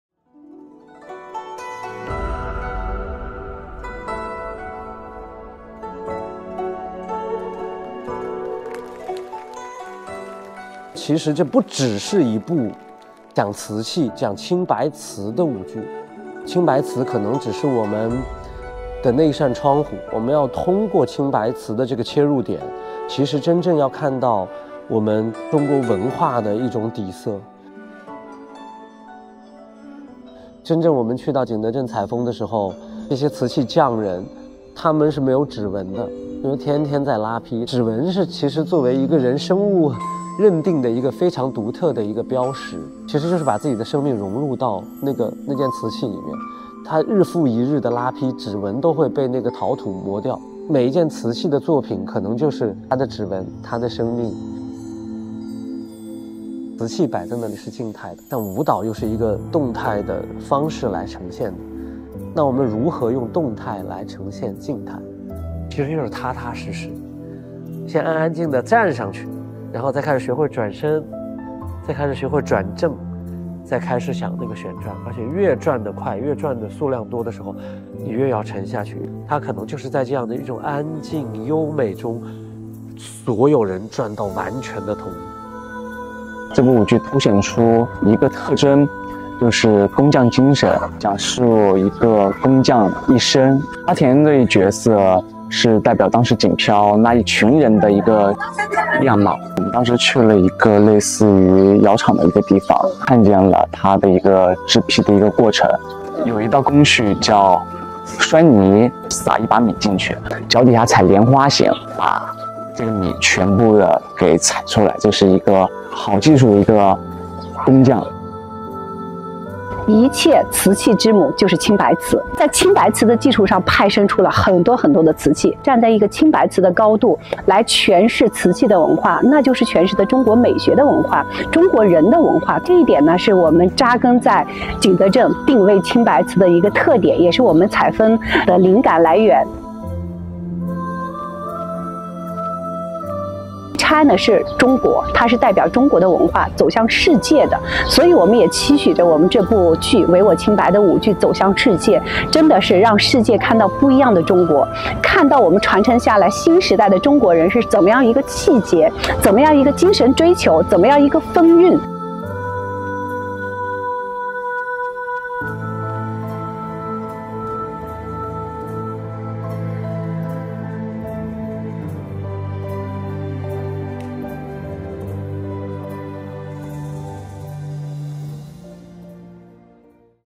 Lyrical dance drama pays tribute to treasured porcelain
In the realm where artistry meets history, Qingbai porcelain of the Song Dynasty (960-1279) emerges as a muse that transcends time. Its delicate beauty, characterized by its translucent glaze and intricate designs, has inspired generations of artists across various mediums.

One such homage comes alive in the form of a mesmerizing dancedrama, where movement, music, and visual art converge to weave a narrative that resonates with the essence of Qingbai porcelain.

Titled "Blue and White: Impression of China", the dance drama, produced and performed by artists of China Oriental Performing Arts Group, will be premiered at Beijing’s Poly Theatre with shows running from Aug 16 to 18.

On July 26, a preview of the dance drama was staged at Beijing’s Drum Tower, one of the landmarks of the capital, marking the opening of the production.